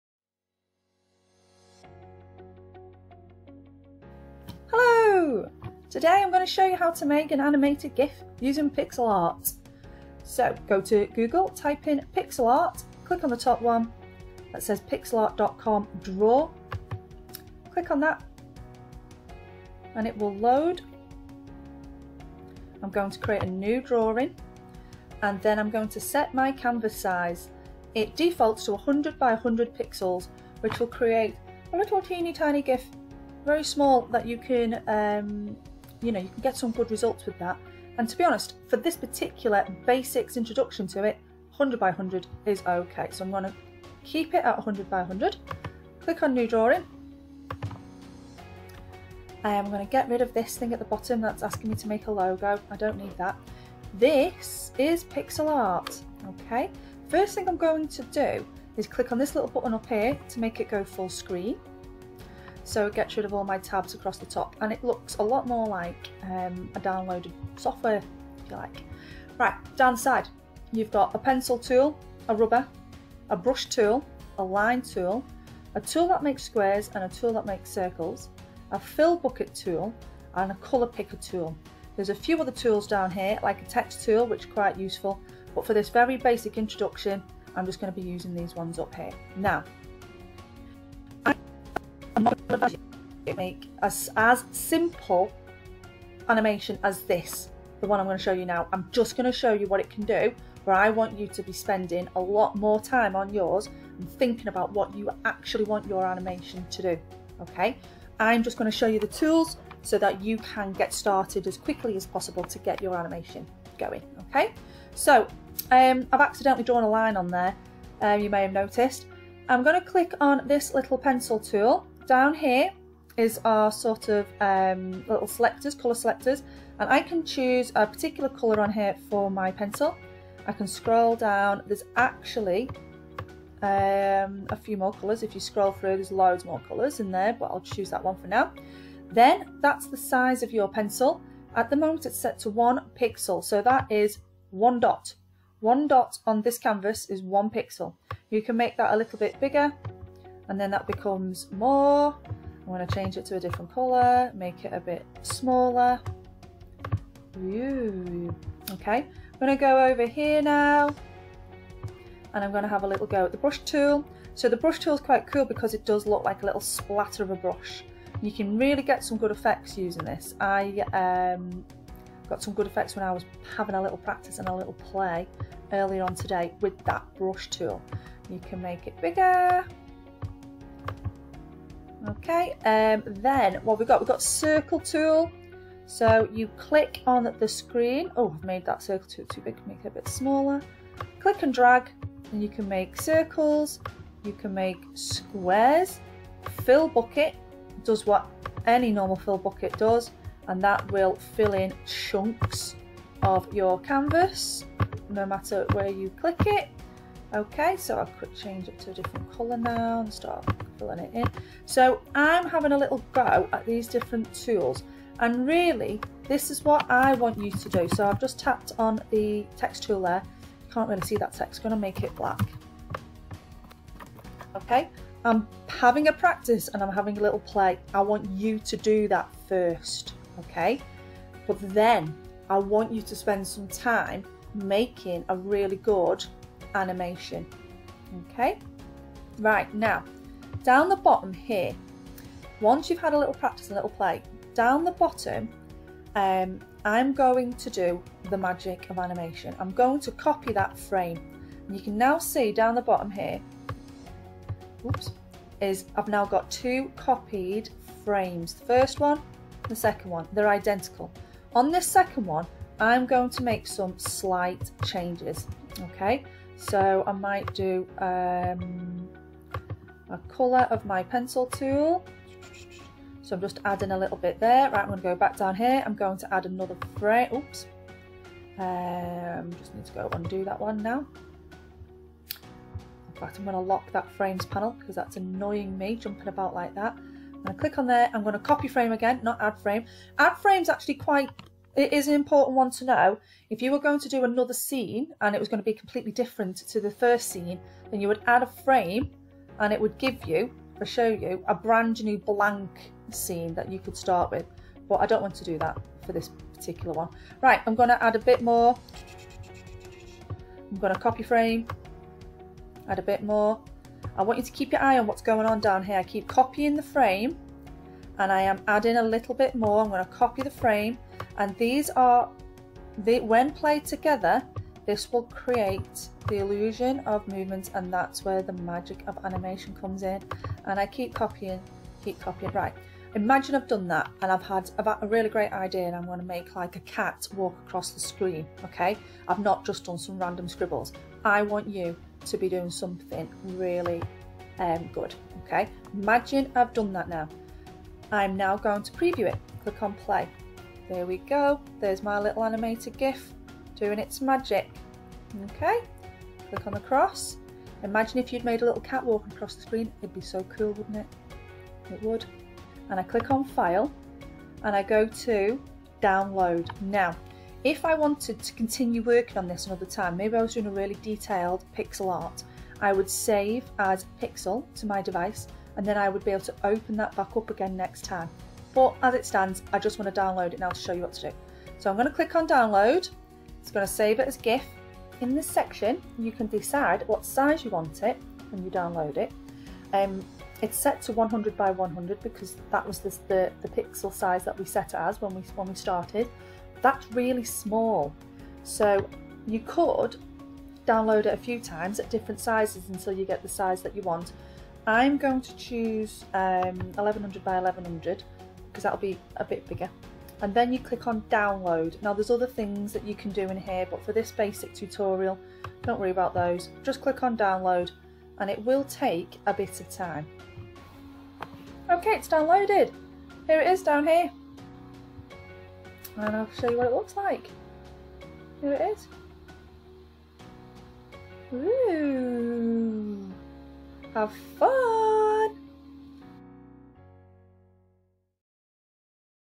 Pixilart 3: Create a cute animated GIF using Pixilart
Absolute beginners guide to creating an animated GIF using Pixilart. Made for those students who are distance learning due to Covid 19.